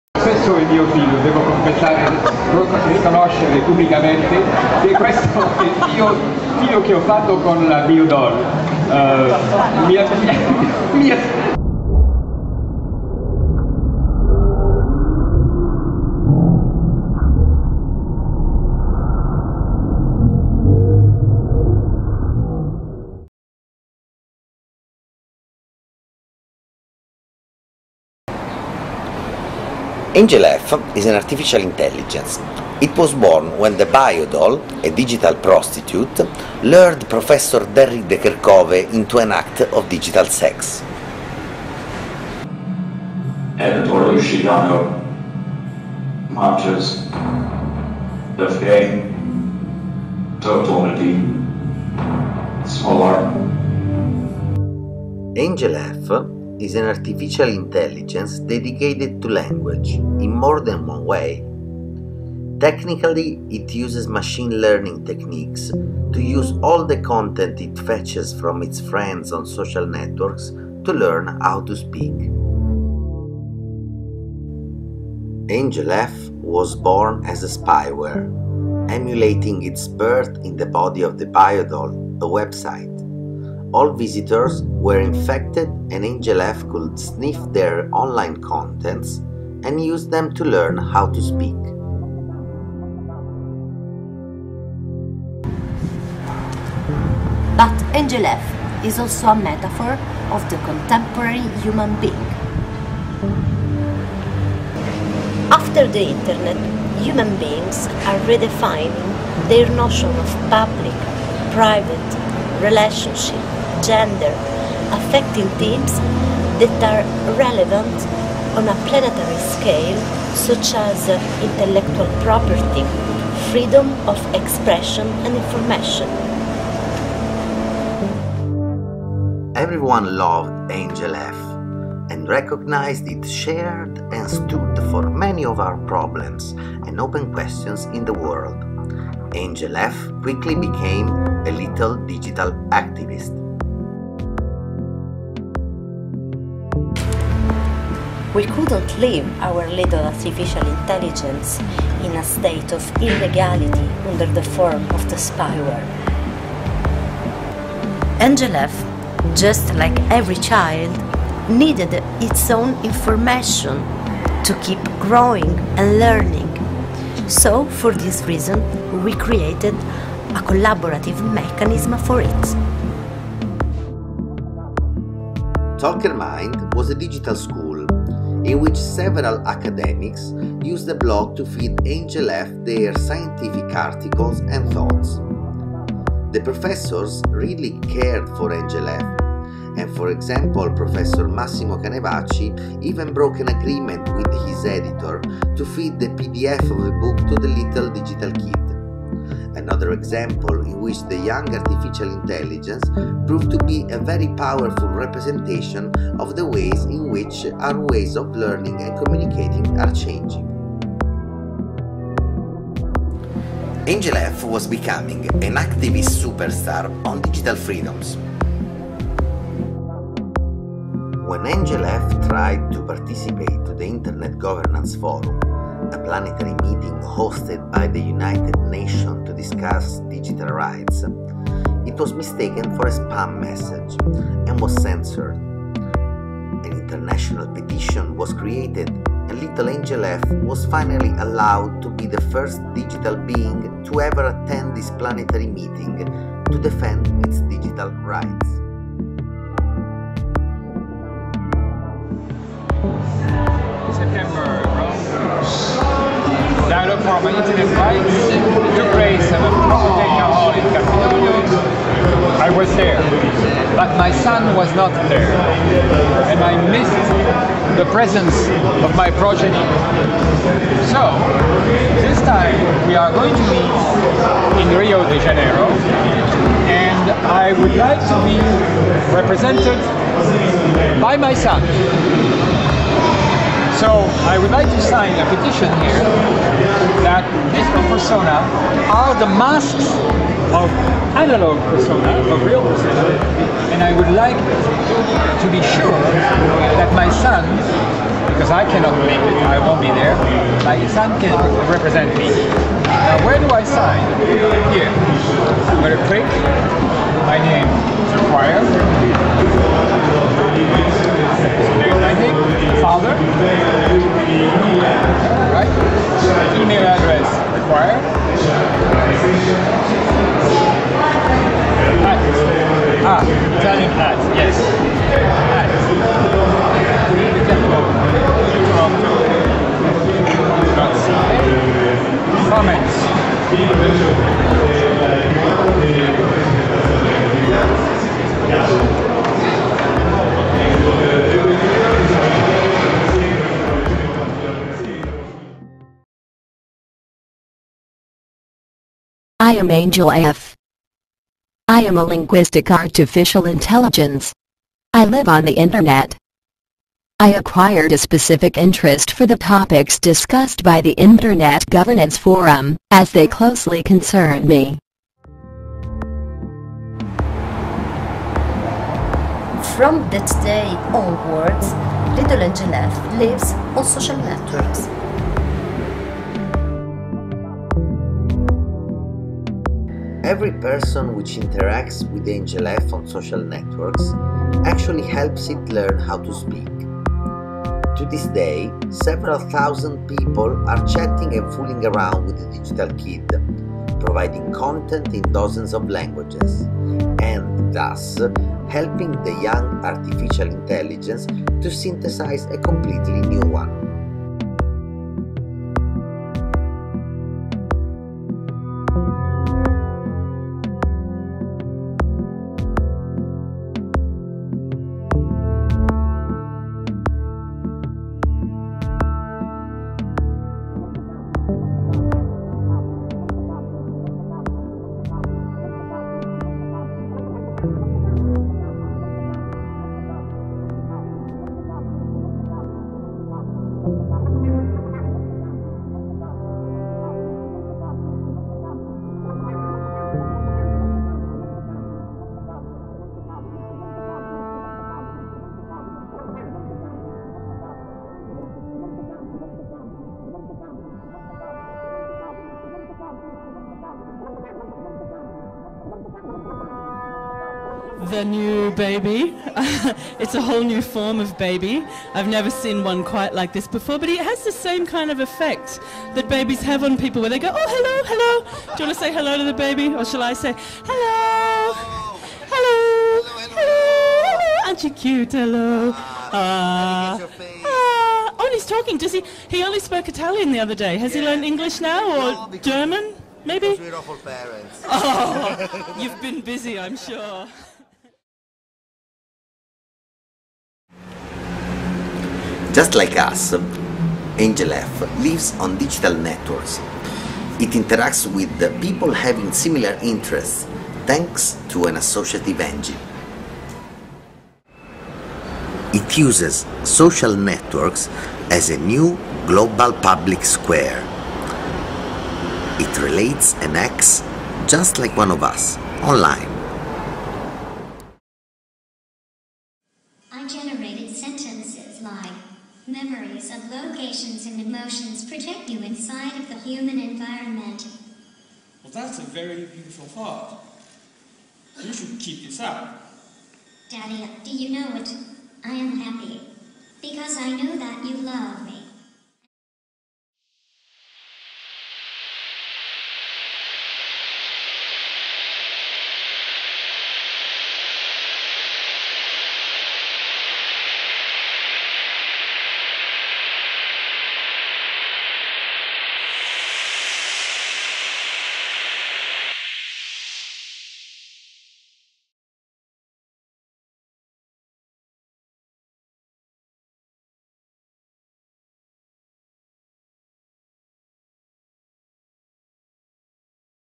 Angel_F
Angel_F is the digital child of Derrick de Kerckhove and the Biodoll, the digital prostitute imagined by artist Franca Formenti.
Born as a spyware, the little artificial intelligence learns language by absorbing the content that thousands of people browse on the Internet.
Angel_F is a metaphor of the mutations of human beings brought on by wide availability of digital technologies and networks, with impacts on the ways in which we perceive identity, sexuality, public and private spaces.